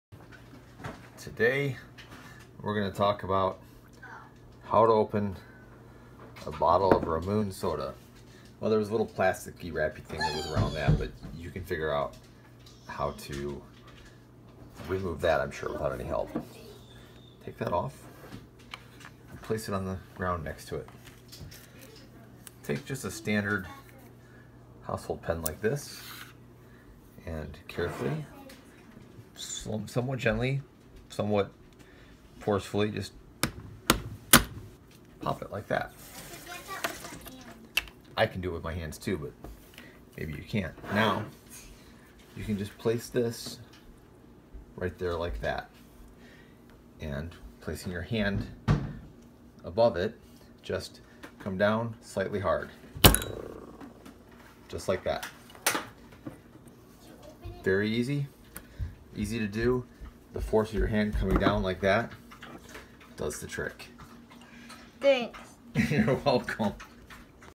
Open a Ramune Soda Bottle
Opening one of these might be a challenge if you've never done it before or seen it done. Here's a video so we don't forget! I may have another video floating around here in how to get the marble out (not easy, unless smashing glass is an option for you, in which case, use a hammer or gently squeeze in your vice)